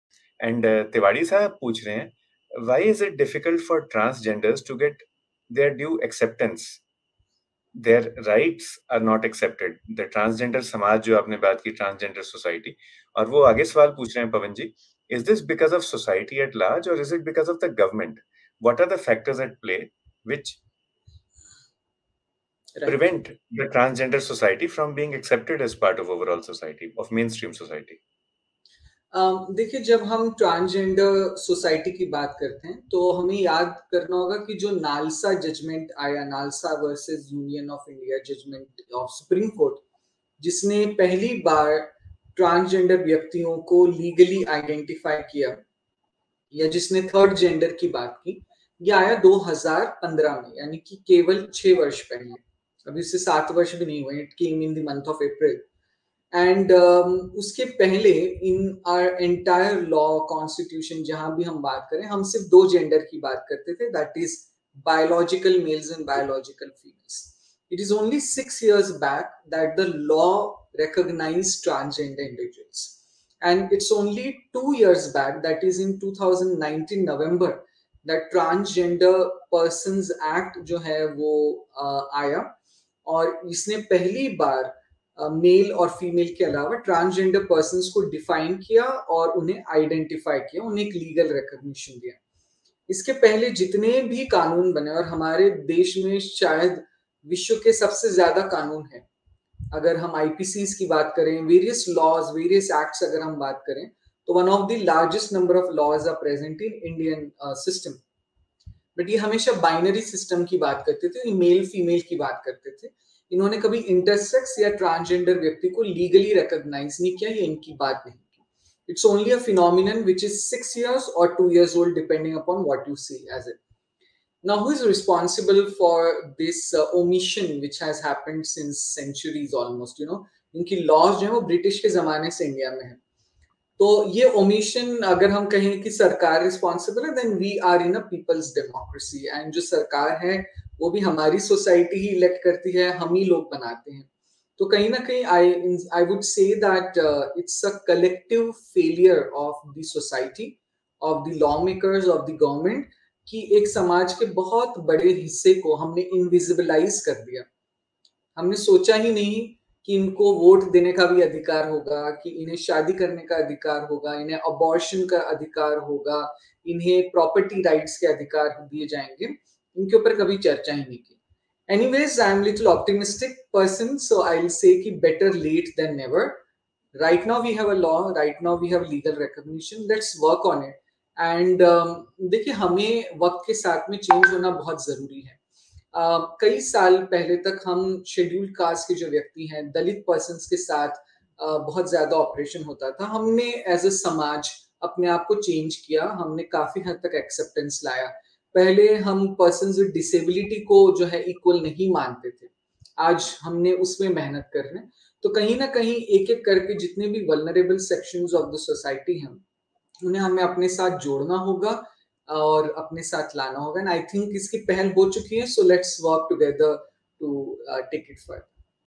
What are the challenges that are faced by the transgender community ( In Hindi ) ?
Dr. Sanjay Kalra Vice President- SAFES, DM Endocrinology, AIIMS New Delhi, FRCP (Edin) talks about